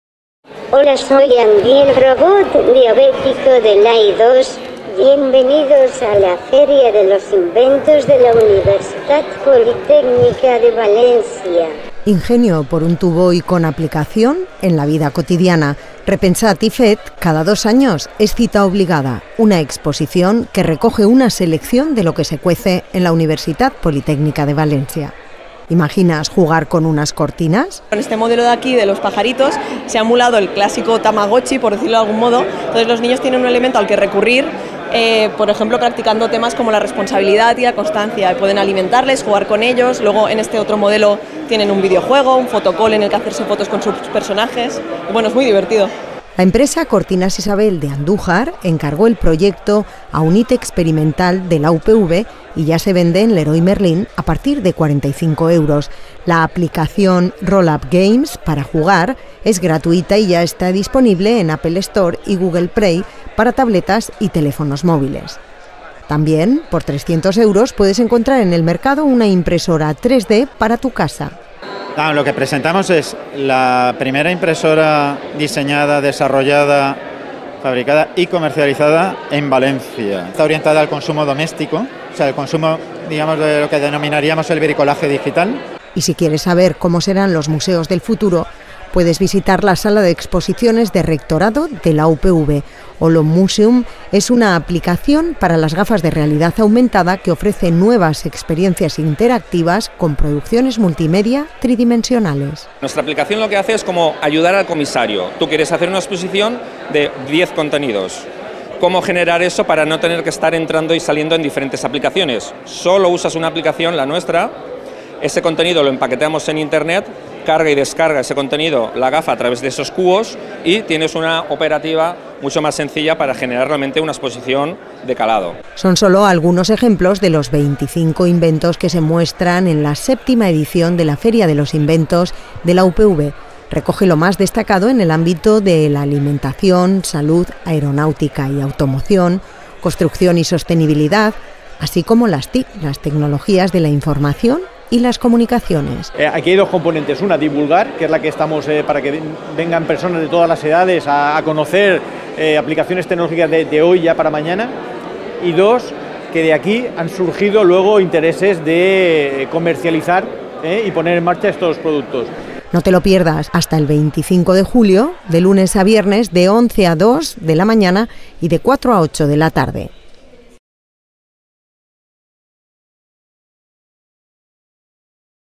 "Repensat i fet". VII Feria de los inventos @UPV - Noticia @UPVTV 13-06-2017
Ingenio por un tubo. Y con aplicación en la vida cotidiana. Repensat i fet, cada dos años, es cita obligada. Una exposición que recoge una selección de lo que se cuece en la Universitat Politècnica de València, ¿Imaginas jugar con unas cortinas?

La empresa Cortinas Isabel de Andújar encargó el proyecto a Unit Experimental de la UPV y ya se vende en Leroy Merlin, a partir de 45 euros. La aplicación Rollup Games para jugar es gratuita y ya está disponible en Apple Store y Google Play, para tabletas y teléfonos móviles.  También por 300 euros puedes encontrar en el mercado una impresora 3D para tu casa.

Y si quieres saber cómo serán los museos del futuro puedes visitar la Sala de Exposiciones de Rectorado de la UPV. Holomuseum es una aplicación para las gafas de realidad aumentada que ofrece nuevas experiencias interactivas con producciones multimedia tridimensionales.

Son solo algunos ejemplos de los 25 inventos que se muestran en la séptima edición de la Feria de los inventos de la UPV. Recoge lo más destacado en el ámbito de alimentación, salud, aeronáutica y automoción; construcción y sostenibilidad, así como las TIC. Tecnologías de la Información y las Comunicaciones.

¡No te lo pierdas! Hasta el 25 de julio, de lunes a viernes, de 11.00 a 14.00 y 16.00 a 20.00 de la tarde.